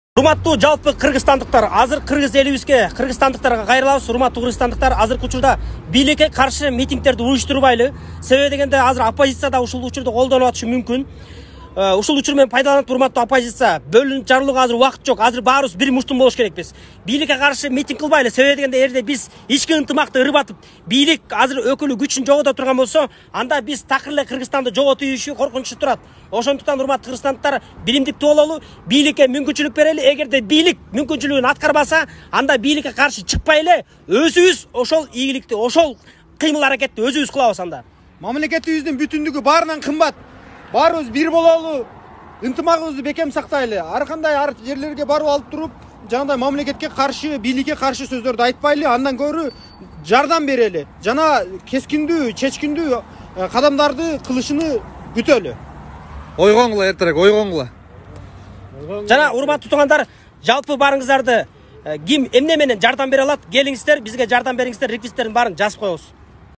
Ички ынтымакты сактайлы!!! Бийлике каршы барбайлы!!! Эгер бийлике каргы чыксак мамлекет алсыздап
Ички ынтымакты сактайлы!!! Бийлике каршы барбайлы!!! Эгер бийлике каргы чыксак мамлекет алсыздап калат жана женилет!!! Ырыс алды ынтыма!!! Ким эмне менен болсода жардам бериниздер

Реквизиттер
ЭЛСОМ / Мбанк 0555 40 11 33
VISA - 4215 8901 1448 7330
ЭЛКАРТ - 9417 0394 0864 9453
Ш.М.А
Жана СБЕРБАНК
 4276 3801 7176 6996